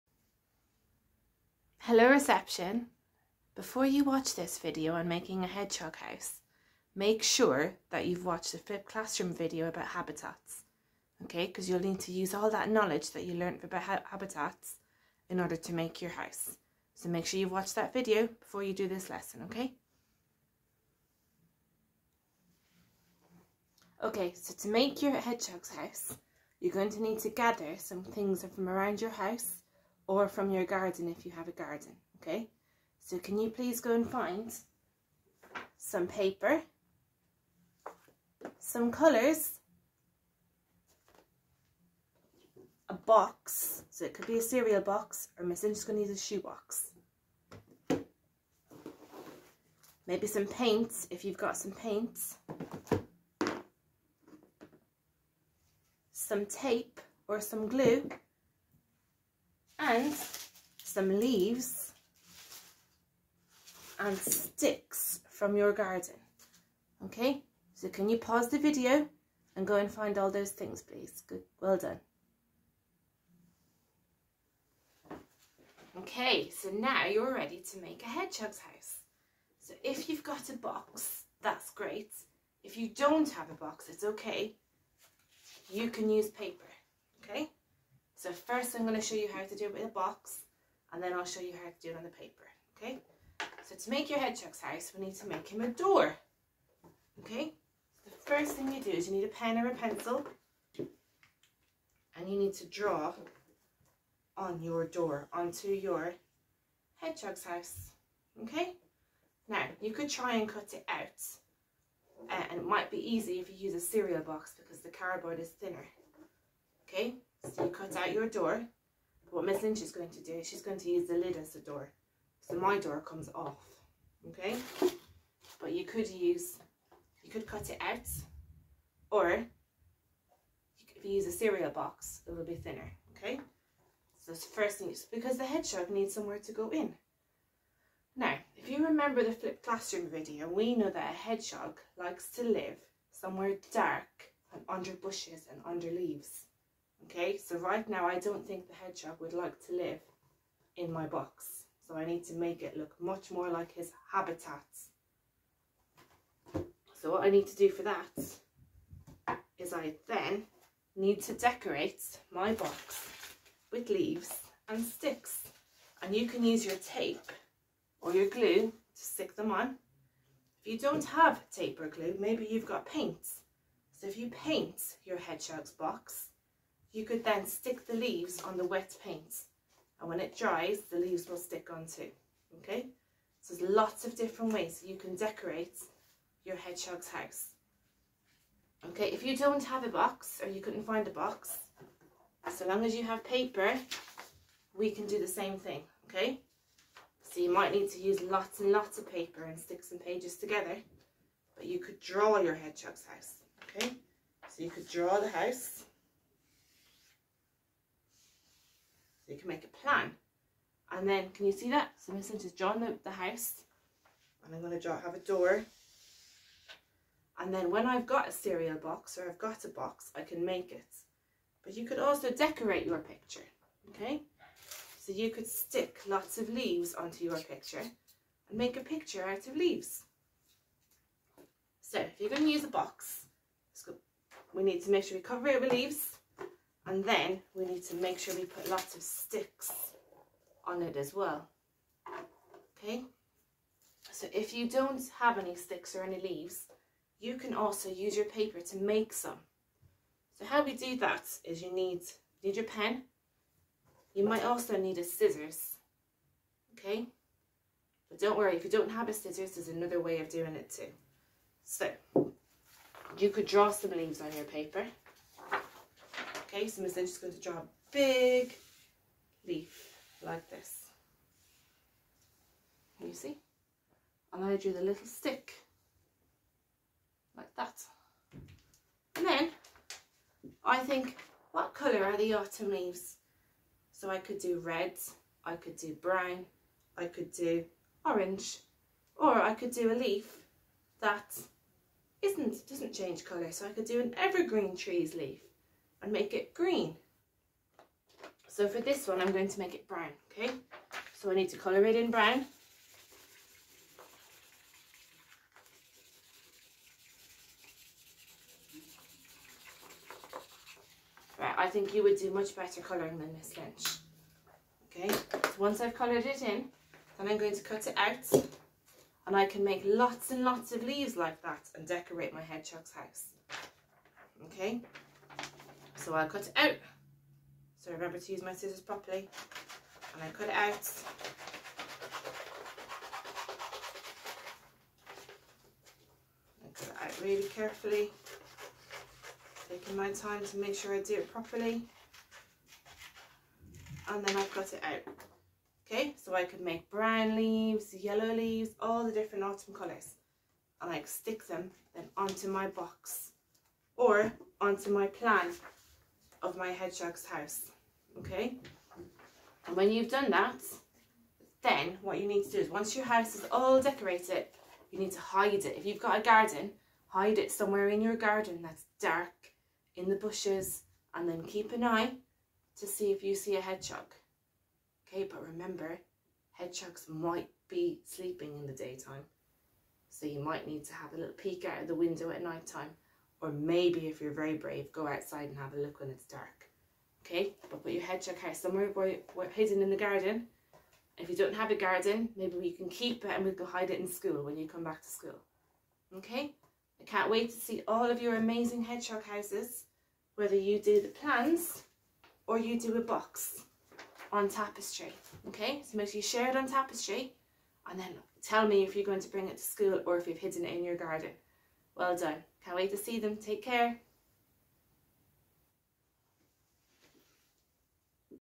Reception 18/11/20 - Afternoon FreeFlow
Make a hedgehog habitat with Miss Lynch.